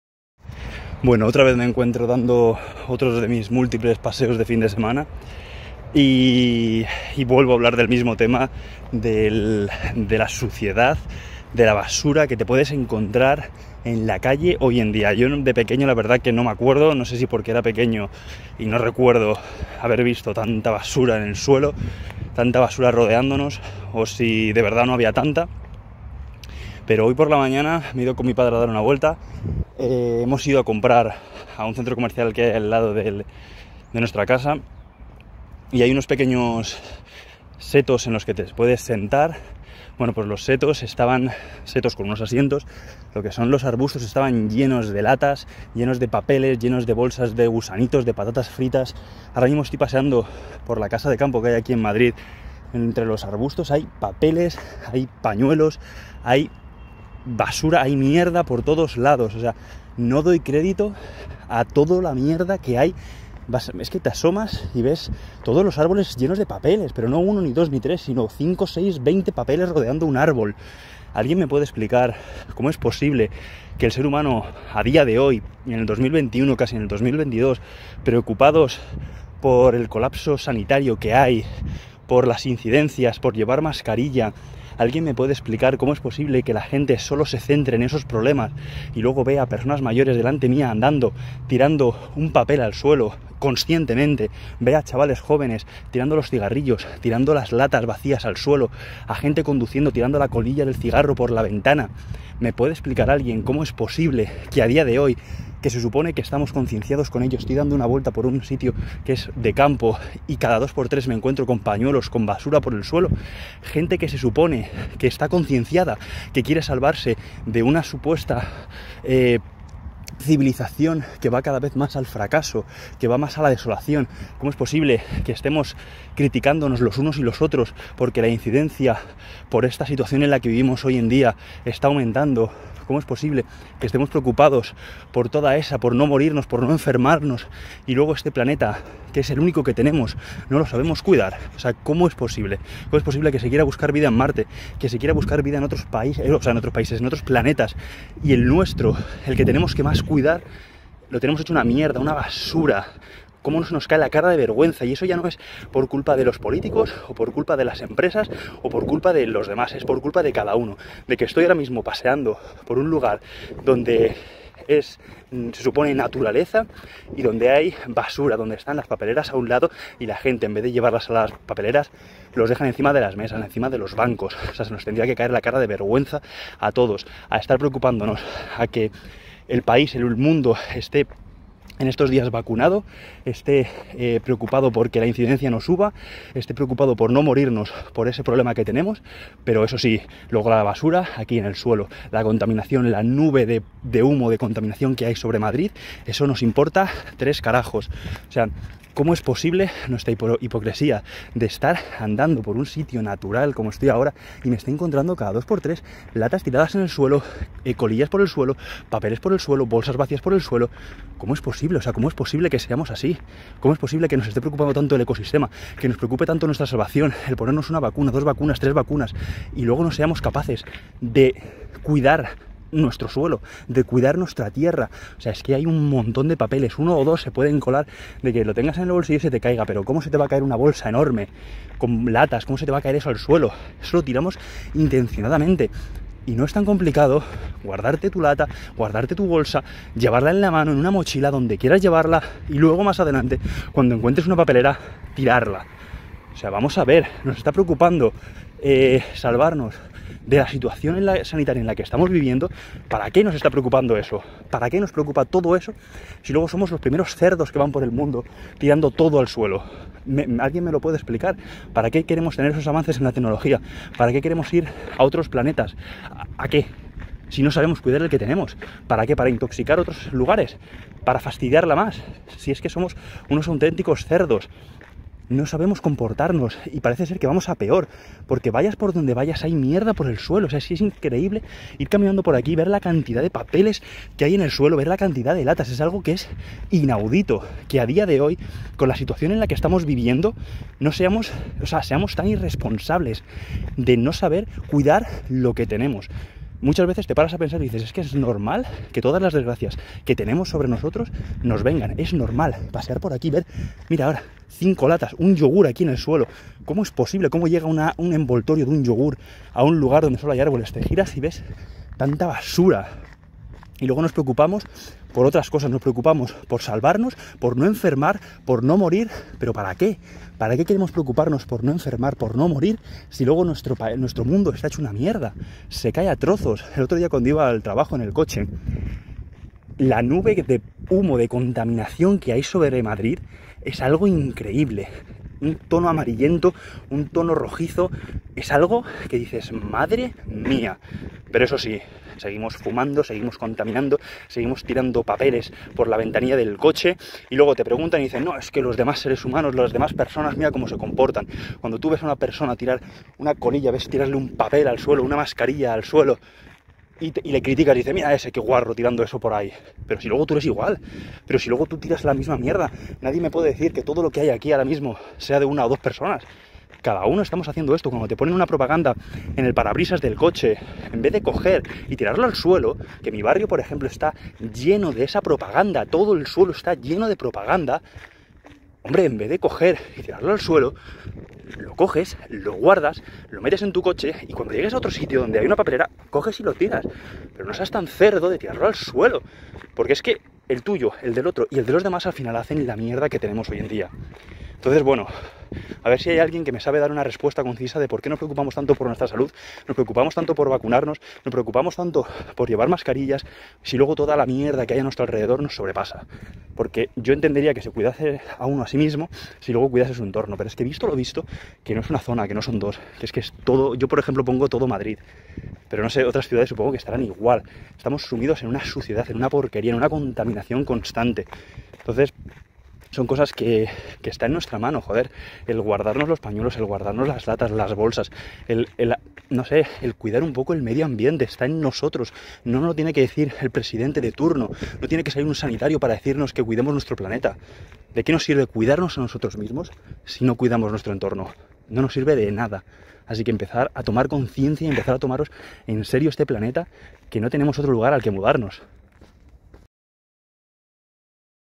SE ACABA EL TIEMPO | CONTAMINACIÓN AMBIENTAL.
Preocupados solo por la sanidad individual dejando de lado nuestra casa, nuestro hogar, el planeta que nos acoge sin pedirnos nada.